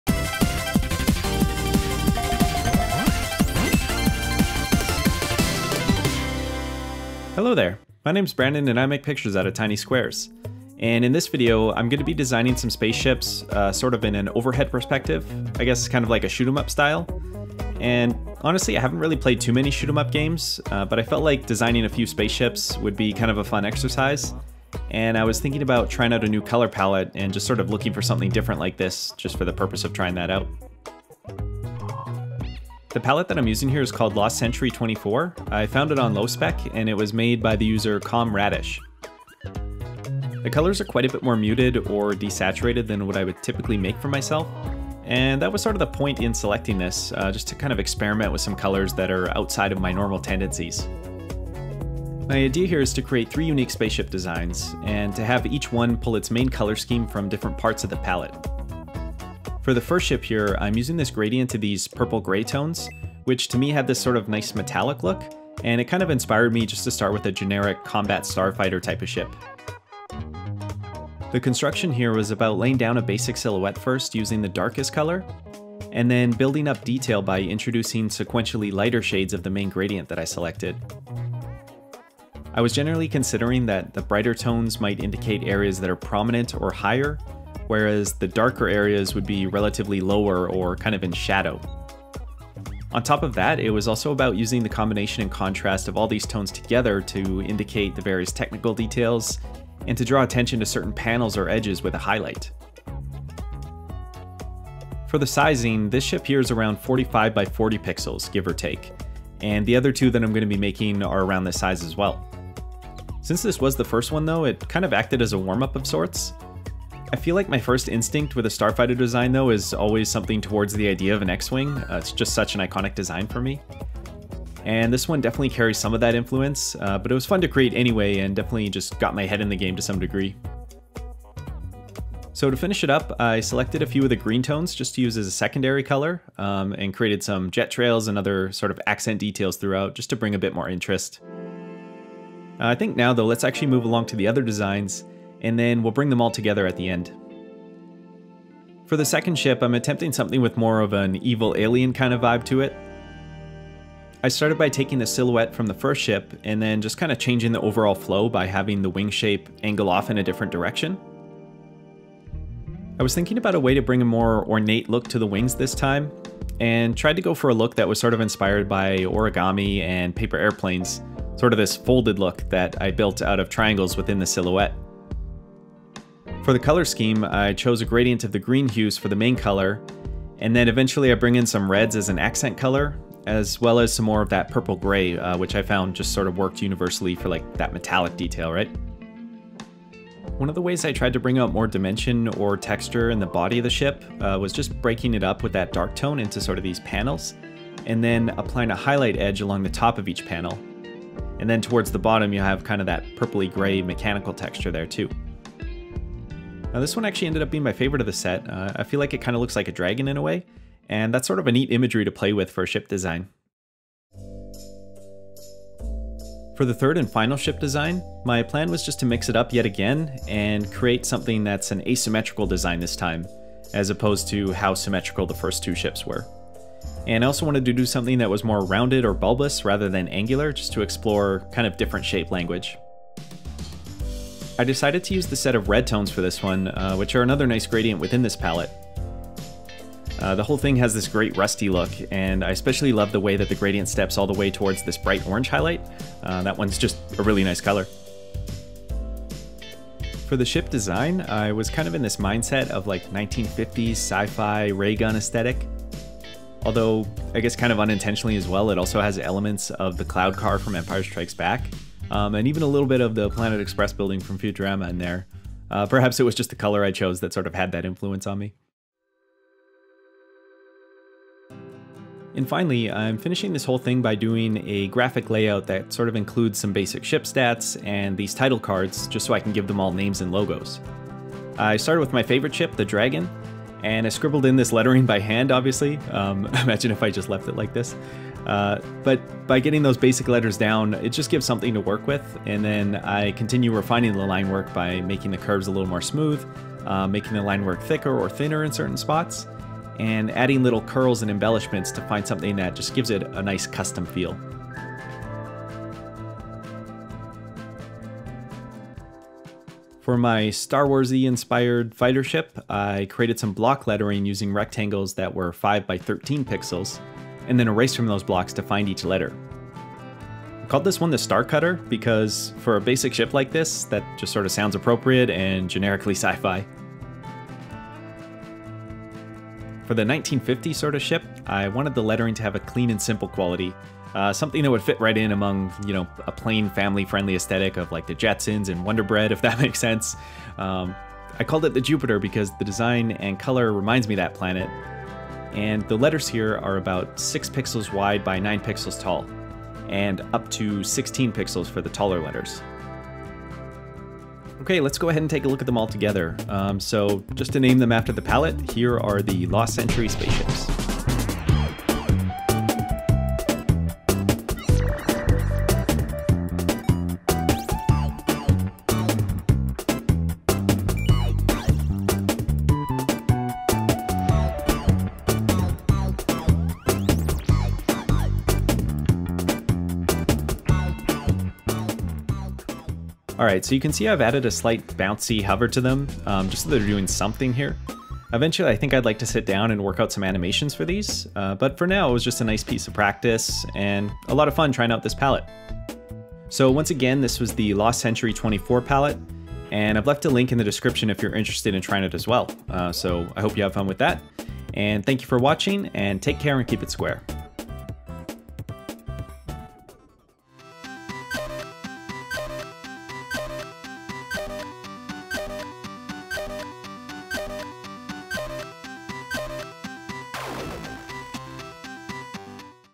Pixel Art Spaceships | LostCentury24 Palette
We're taking to the stars in this timelapse, creating a few top-down-style pixel art spaceship designs. I try out the lostcenturypaletteV2, and discuss some of my design choices for each ship!

0:00 INTRO
0:06 SHIP 1
2:28 SHIP 2
3:36 SHIP 3
4:31 TITLE LETTERING
5:54 FINAL ART
6:50 VID OVER